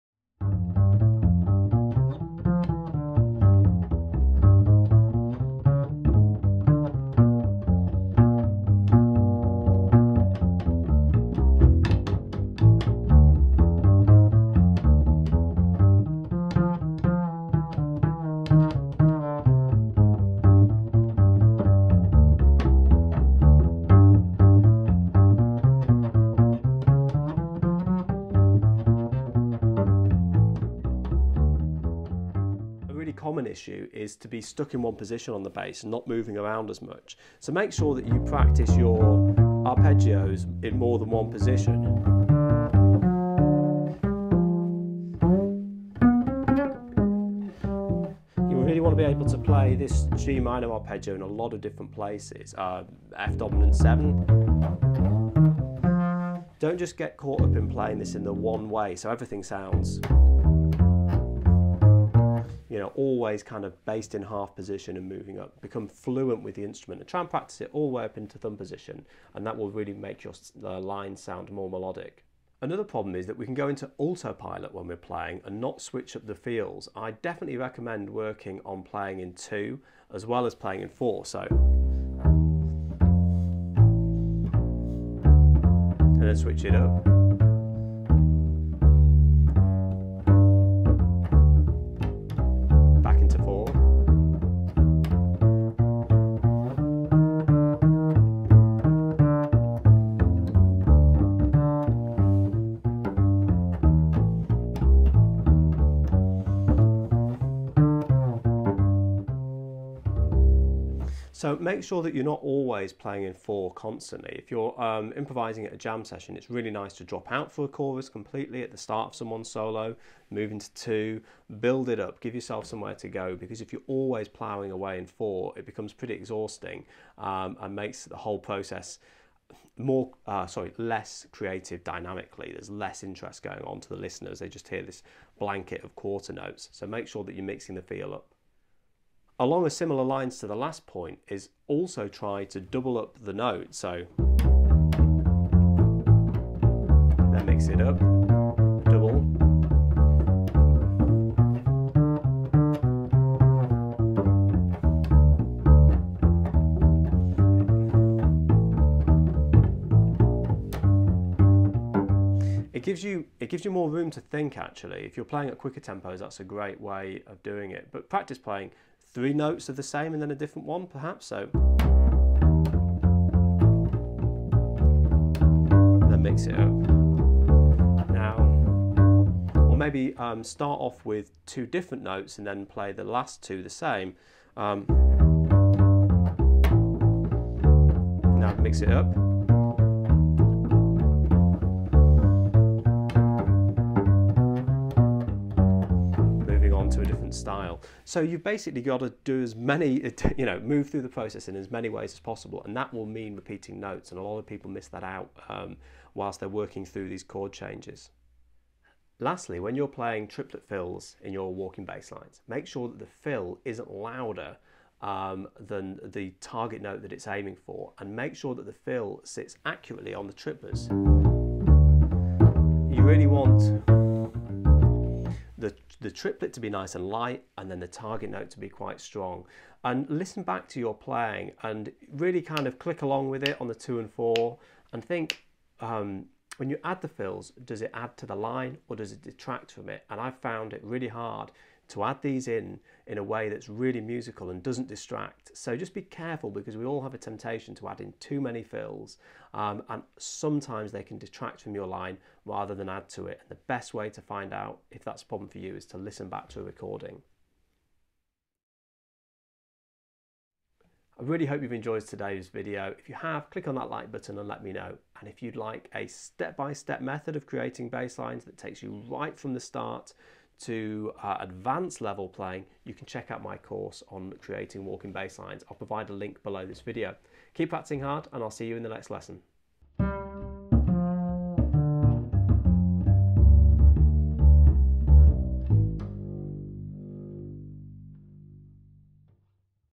Common Problems with Walking Bass Lines Part 2
- All our lessons & courses in one place + free double bass player resources!

In this video, I explore the most common problems I see with walking bass lines and give advice on how to avoid them. This is the second part of the lesson and if you want to check out the 1st part here's a link:

I hope you enjoy the video. What areas did you find challenging?
Best of luck with your practice, cheers Geoff